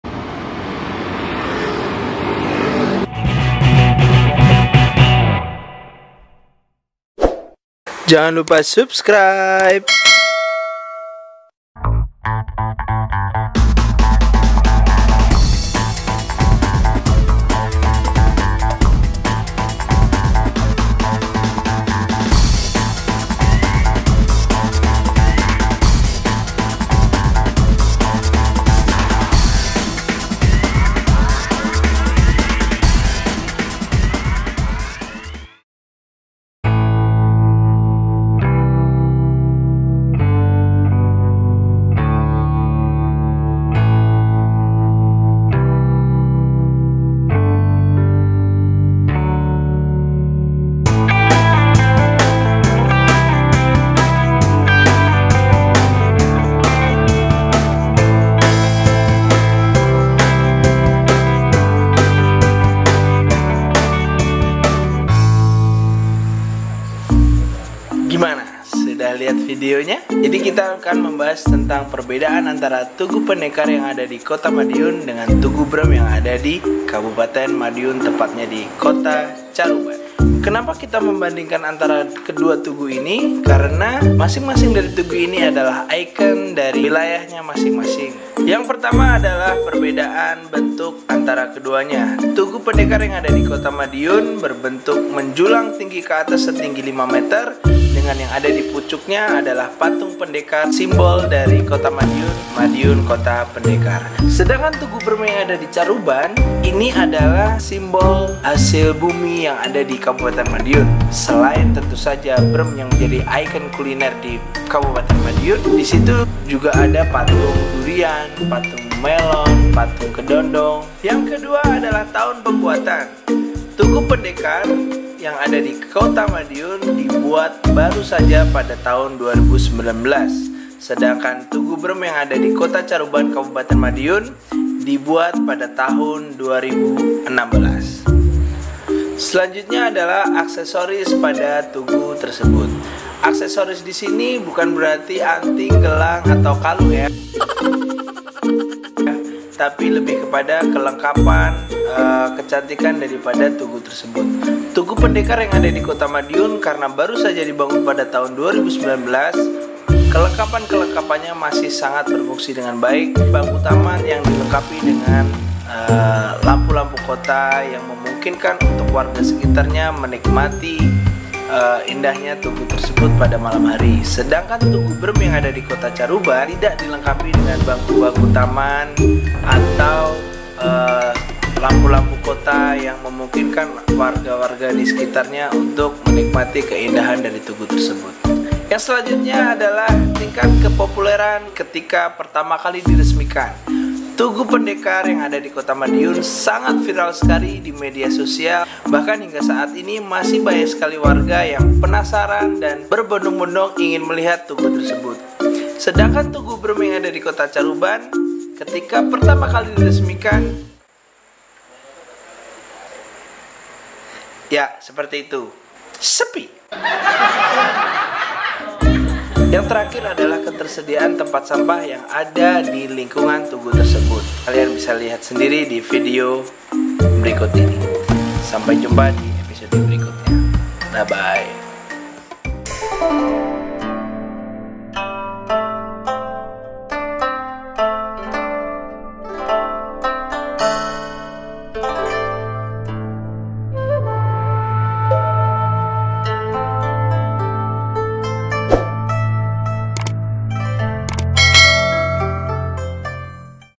TUGU PENDEKAR VS TUGU BREM
Kira-kira, apa saja perbandingan antara Tugu Pendekar dengan Tugu Brem? Tonton videonya sampai habis!

CarubanID bisa kamu temui disini